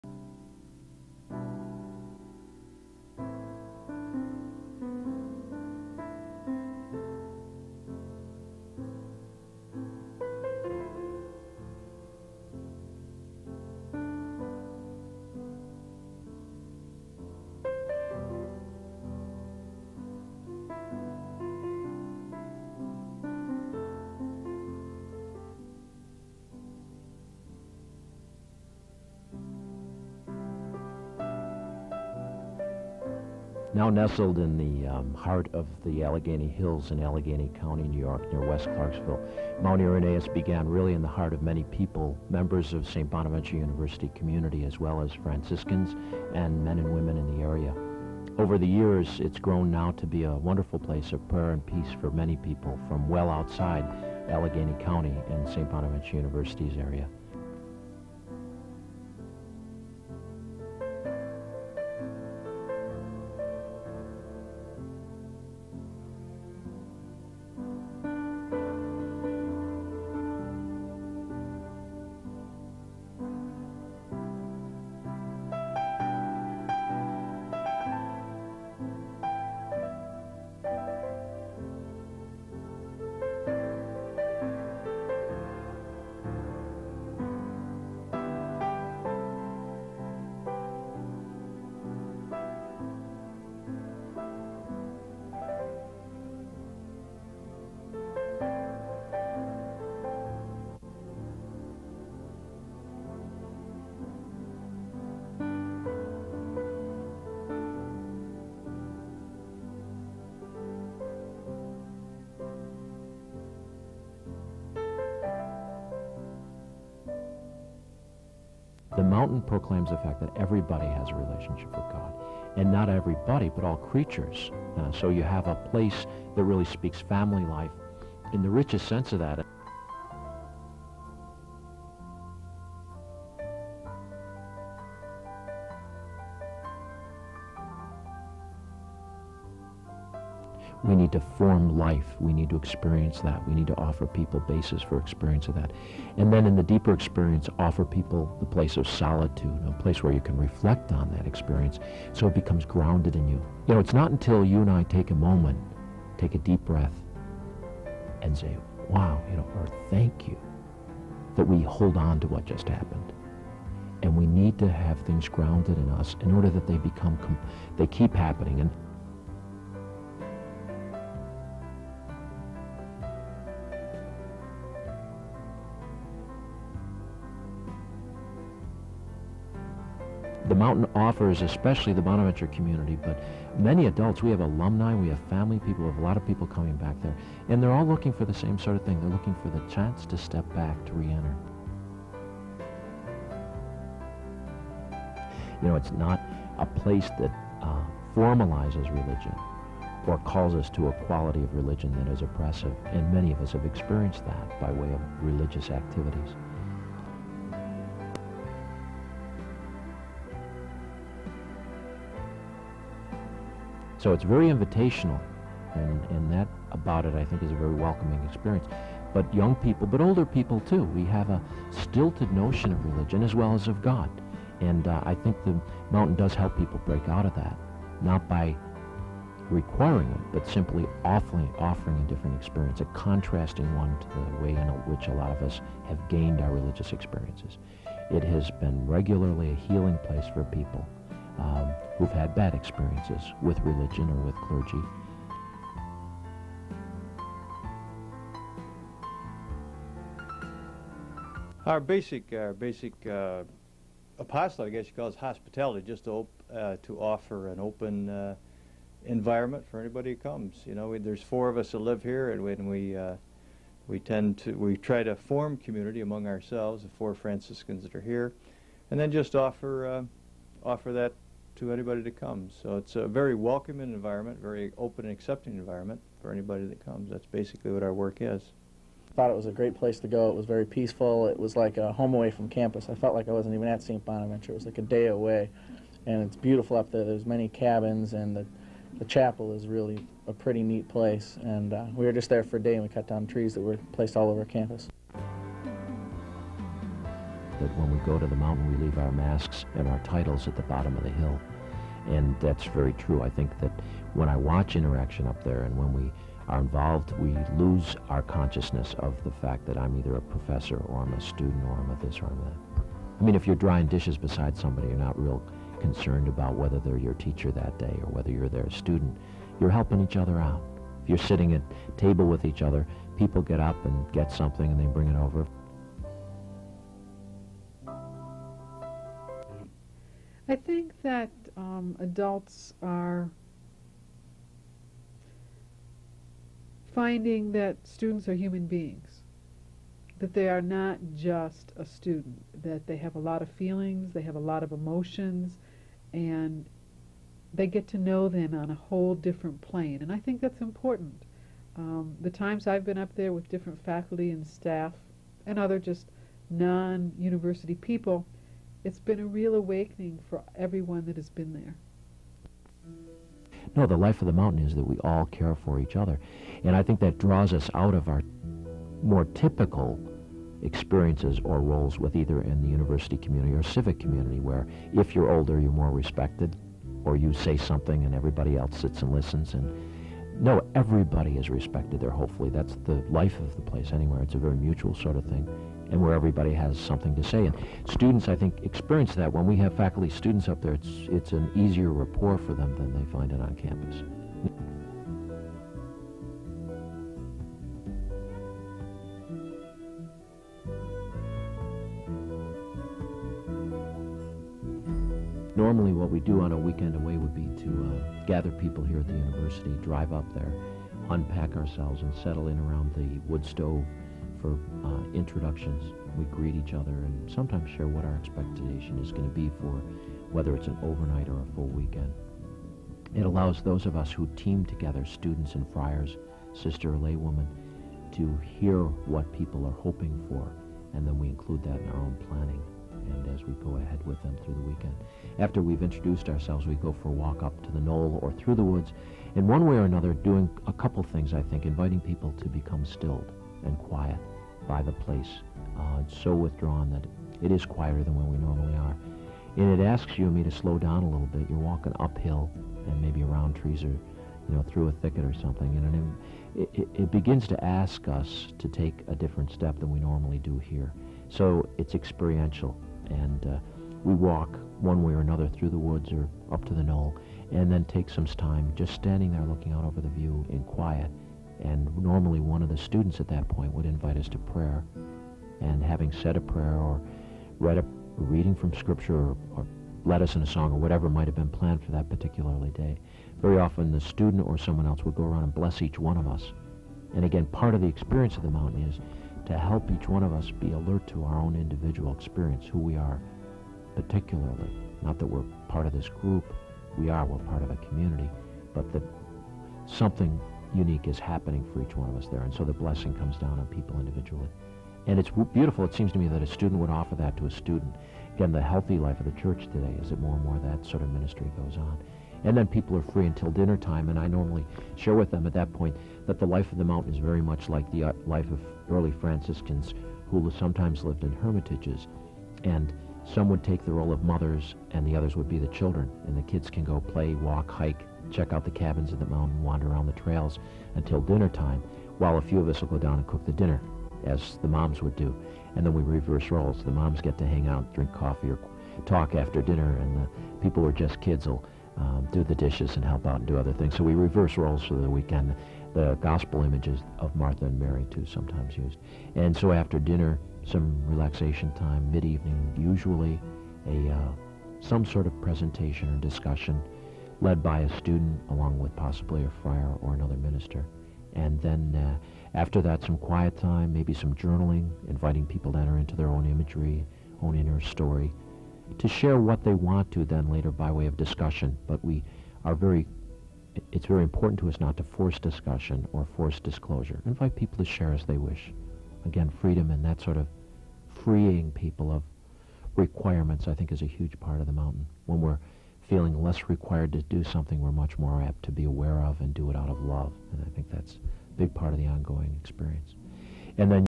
Climbing Mt. Irenaeus - The Early Years (Part 1 of 2)
This video, made sometime in the early 1990s, documents early life at Mt. Irenaeus. Fr. Dan Riley, OFM, narrates.